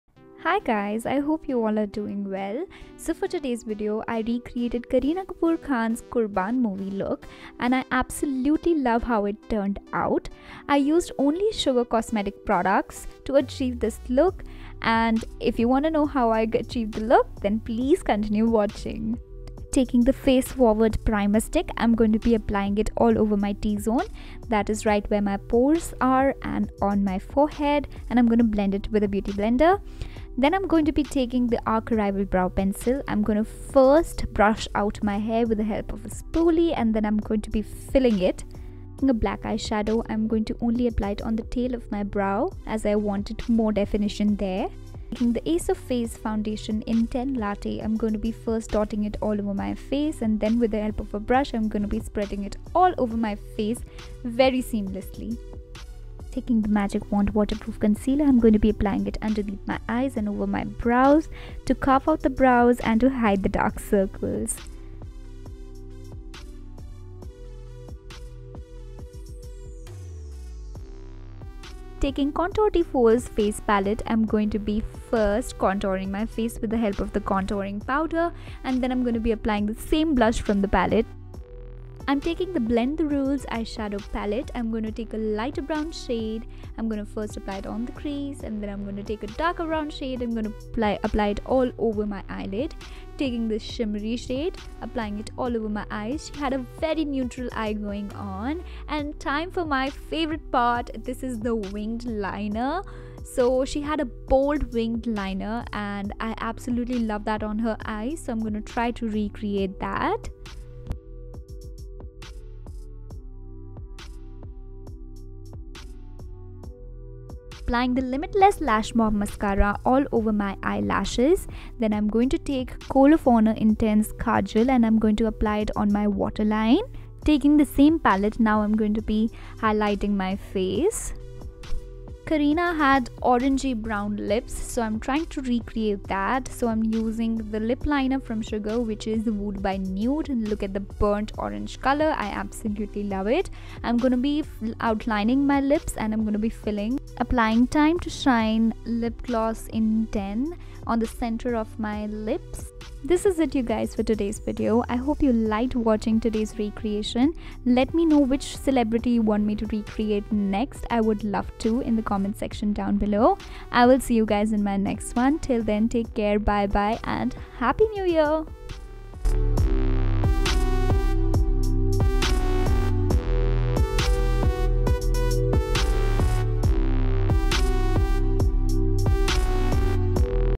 KAREENA KHAN KAPOOR KURBAAN MOVIE INSPIRED LOOK | CELEBRITYRECREATION.
Hi Glow fam! wishing you a very happy new year. 🥳
you guys love watching my recreation videos, so I thought of coming up with a one brand recreation video using all @SUGARCOSMETICS PRODUCTS.